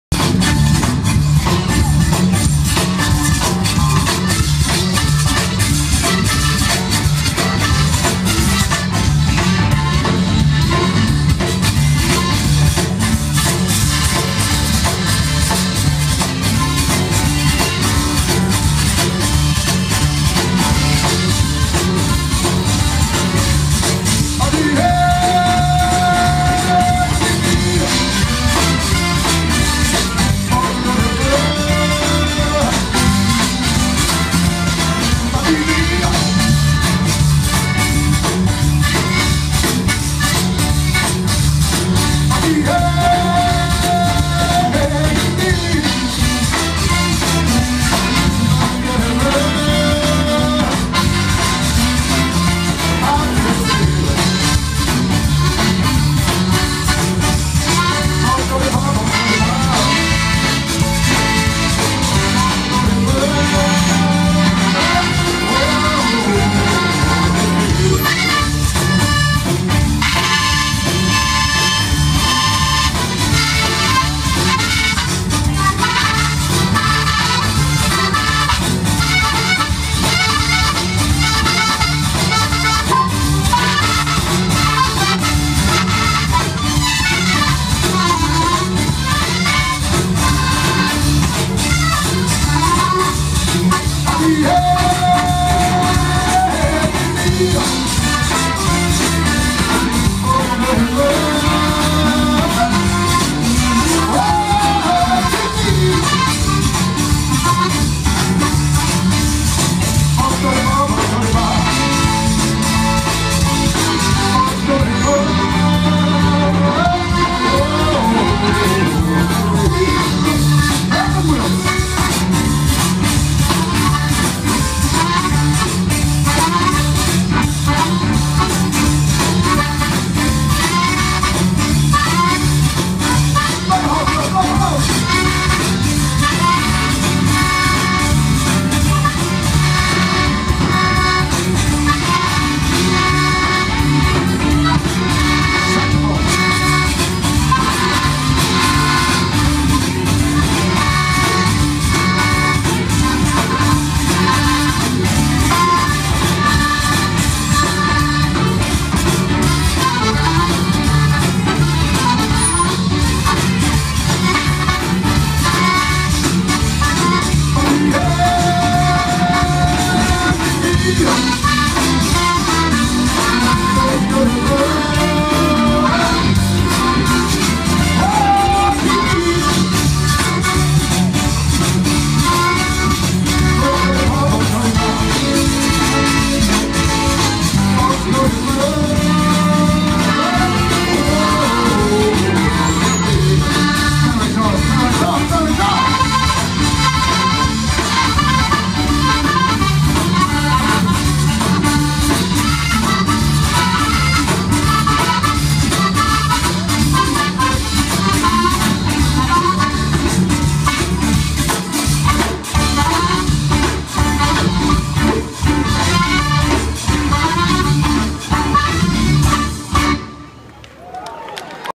Rockin' Dopsie Jr & The Zydeco Twisters, April 19. 2014
Sugar Mill Pond, Yongsville, Lafayette, April 19. 2014